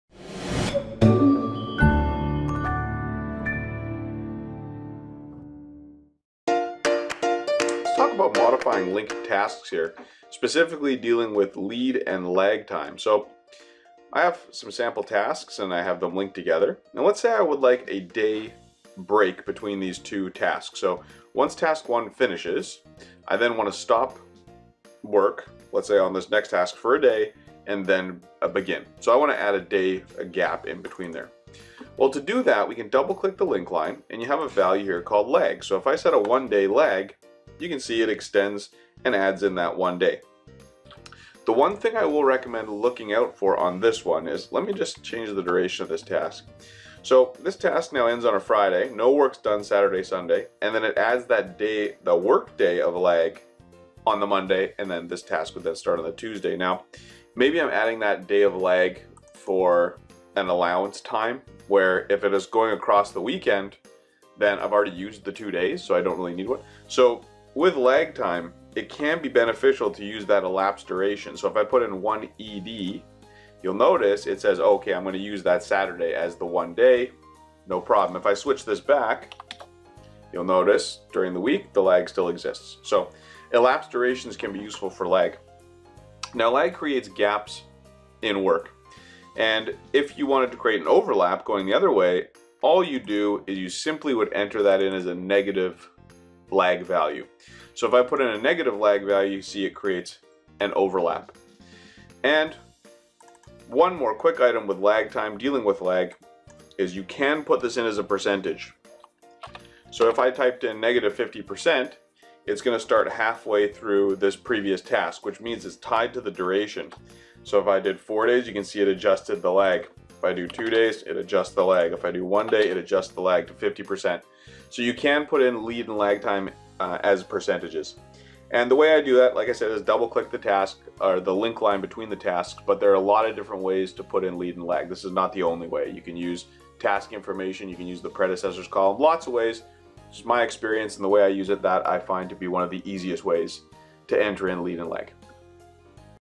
Microsoft Project - Setting Lead and Lag Time
Now that we know how to create links we need to know how to add gaps or overlaps of time.  This time is called Lead (overlap) and Lag (gap).